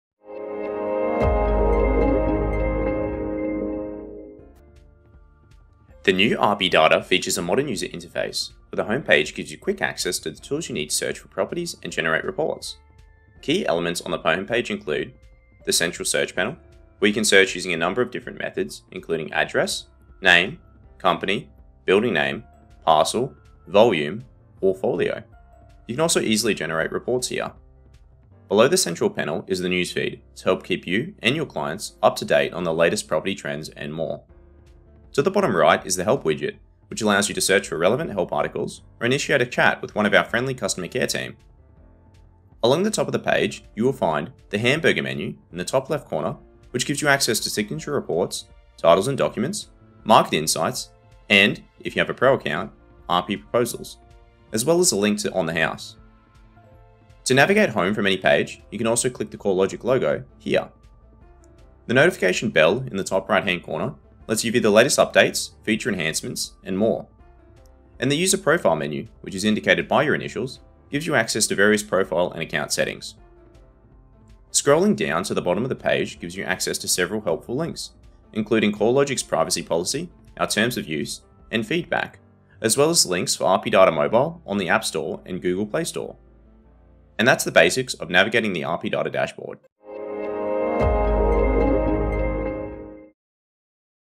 Navigating the Homepage in RP Data
Learn how to navigate the homepage in RP Data to efficiently access key features and tools. For more helpful information, visit the CoreLogic help centre